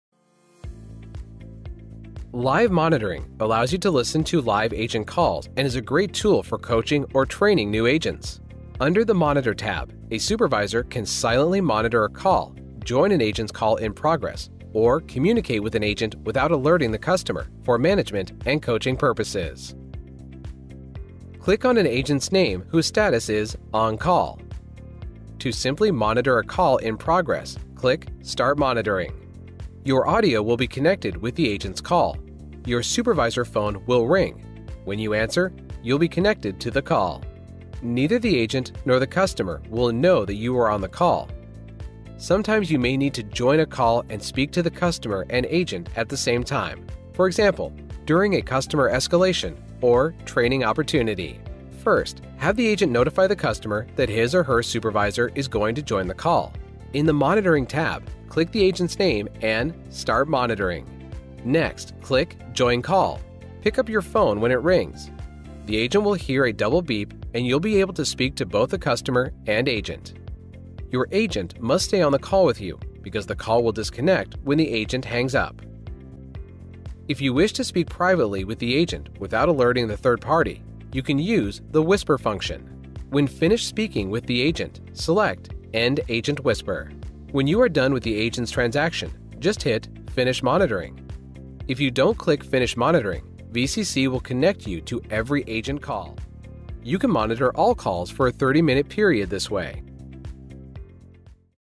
Live Monitoring in 8x8 Contact Center
In this video learn how to use Live Monitoring which allows you to listen to live agent calls.

Timestamp:
0:20 Start monitoring
0:42 Join a call
1:17 Speak with the agent
1:27 End monitoring